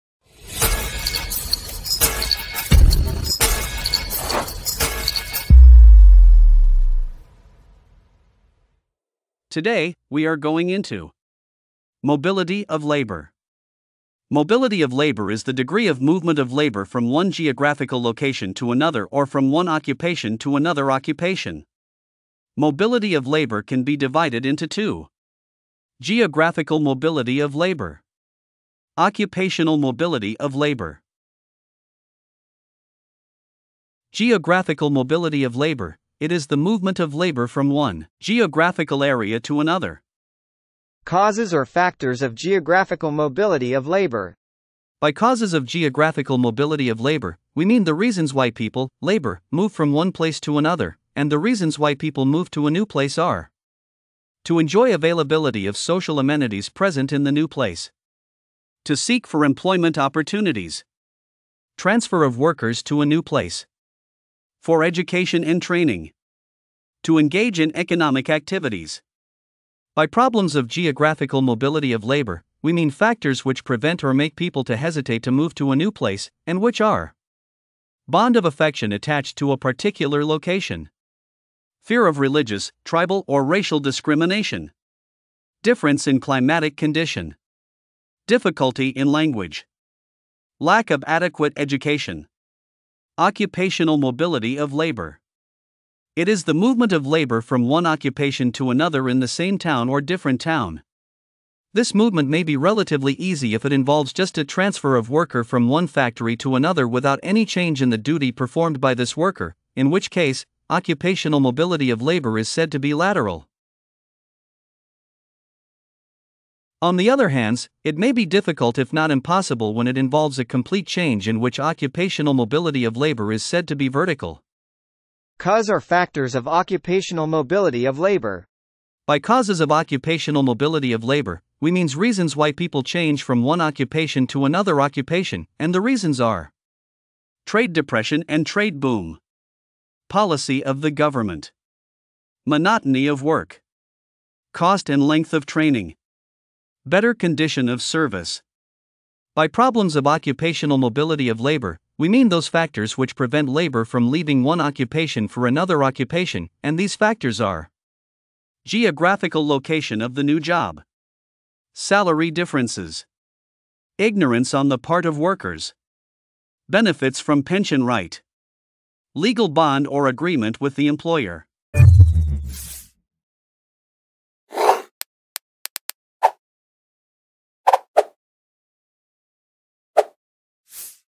Unlocking Labour Mobility: Key Factors and Challenges Explained
Mobility of Labour refers to the ability of workers to move from one geographical location to another or switch from one occupation to another. This mobility is crucial for optimizing resource allocation in an economy, reducing unemployment, and enhancing overall productivity.

Types of Labour Mobility
Geographical Mobility of Labour:

This is the movement of workers from one geographic area to another. It can be within a country or across countries.
Occupational Mobility of Labour:

This involves workers changing their occupation or job role, either within the same town or to a different town.
Causes/Factors of Geographical Mobility:

Availability of Social Amenities:

Better healthcare, education, housing, and recreational facilities can attract workers to new locations.
Employment Opportunities:

Regions with higher job availability and better employment prospects draw workers seeking jobs.
Transfer of Workers:

Companies may transfer employees to different branches or locations.
Education and Training:

Pursuit of better education and training opportunities can motivate relocation.
Economic Activities:

Engaging in different or more lucrative economic activities can prompt geographical movement.
Problems of Geographical Mobility:

Bond of Affection:

Emotional attachment to a particular location can hinder movement.
Fear of Discrimination:

Concerns about facing religious, tribal, or racial discrimination can deter relocation.
Climatic Conditions:

Differences in climate between regions can make relocation less appealing.
Language Barriers:

Difficulty in communicating in a new language can be a significant deterrent.
Lack of Adequate Education:

Insufficient education can limit the ability to find suitable employment in a new location.
Causes/Factors of Occupational Mobility:

Trade Depression and Boom:

Economic downturns (depressions) and upswings (booms) can cause workers to switch occupations.
Government Policies:

Changes in regulations or policies can incentivize or force workers to change jobs.
Monotony of Work:

Repetitive and uninteresting jobs can drive workers to seek new occupations.
Cost and Length of Training:

The feasibility of acquiring new skills through training can affect occupational mobility.
Better Conditions of Service:

Improved salary, benefits, and working conditions can attract workers to new occupations.
Problems of Occupational Mobility:

Geographical Location:

The location of new job opportunities can be a barrier if they require relocation.
Salary Differences:

Differences in pay between current and potential new jobs can affect the decision to switch occupations.
Ignorance:

Lack of awareness about available opportunities can limit occupational mobility.
Pension Rights:

Benefits tied to current employment, such as pensions, can deter job changes.
Legal Bonds:

Contracts and agreements with current employers can restrict the ability to move to new occupations.
Conclusion
Understanding the factors influencing the geographical and occupational mobility of labour is essential for formulating policies that enhance labour market efficiency. By addressing the challenges and leveraging the drivers of labour mobility, economies can better match labour supply with demand, improve employment rates, and optimize resource utilization.

Mobilizing labour effectively can significantly impact economic growth and individual livelihoods.